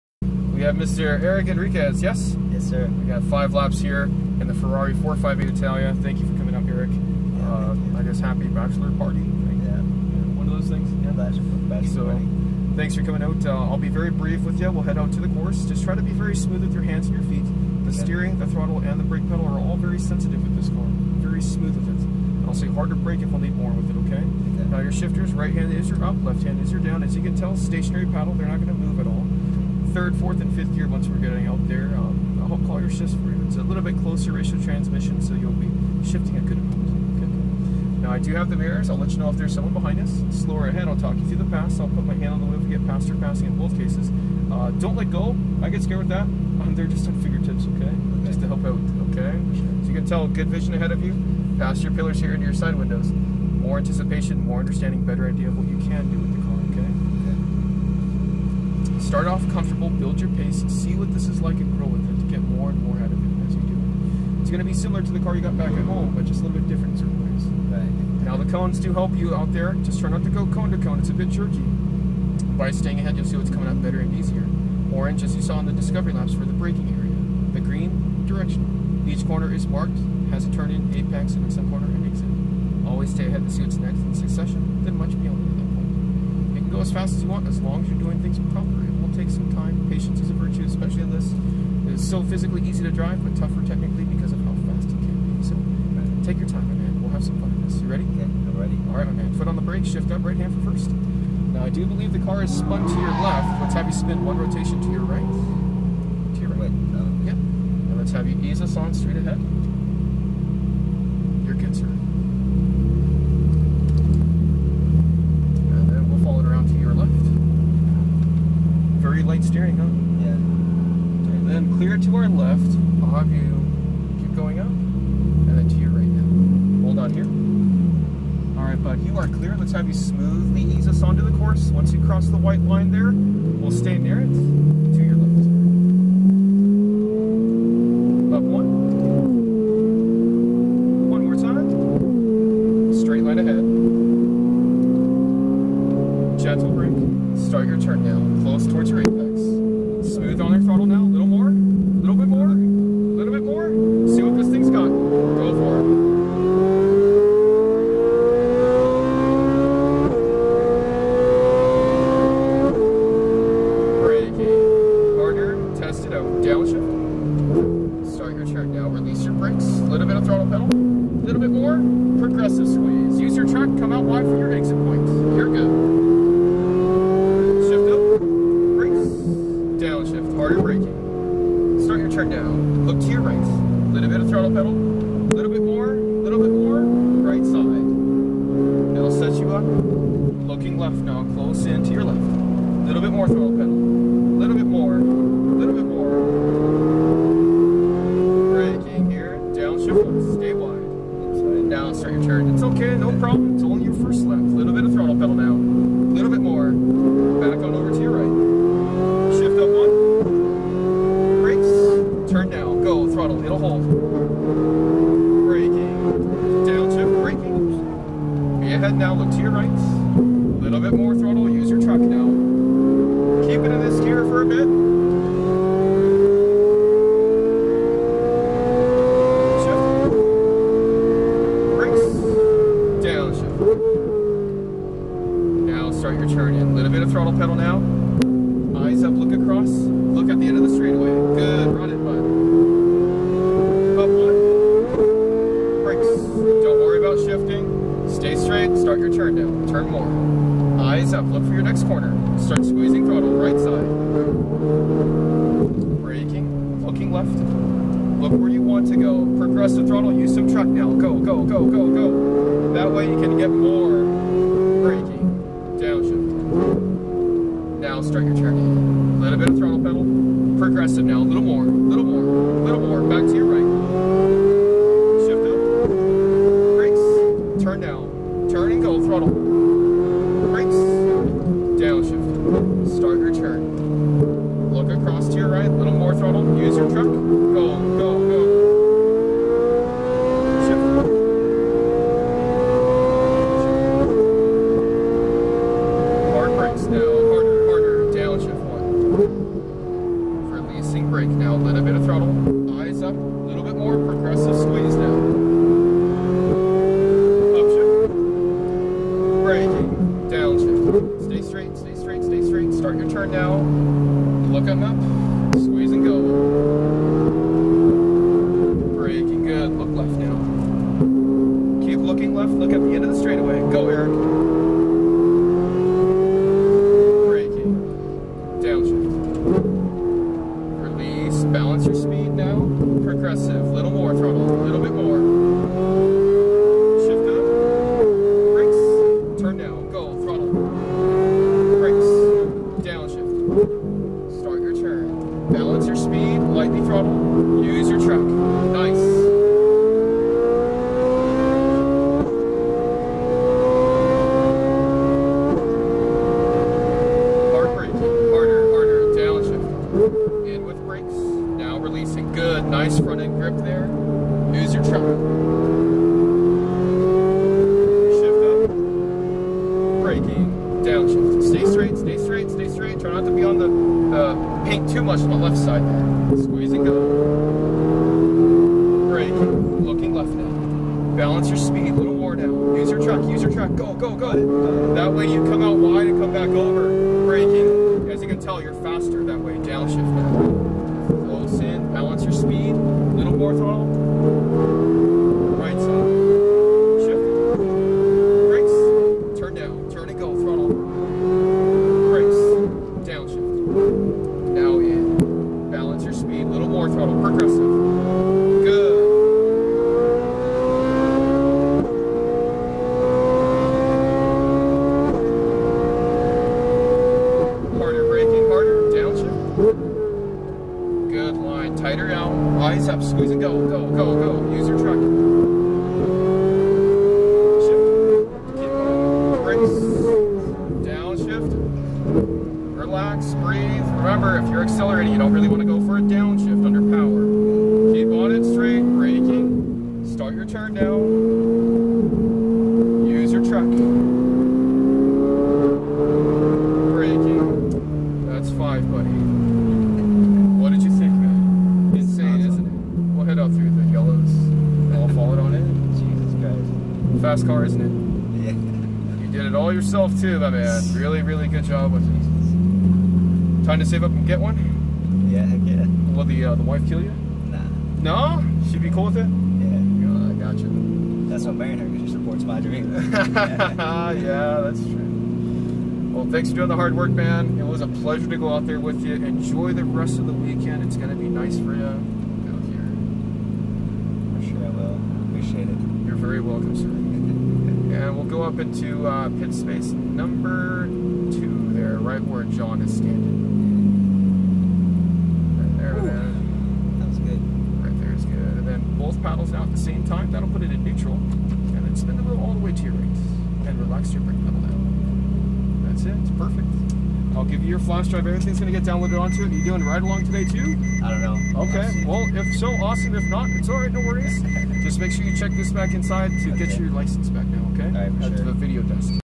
Eric Enriquez in a Ferrari 458 Italia
Bachelor party fun at the Las Vegas track driving exotics.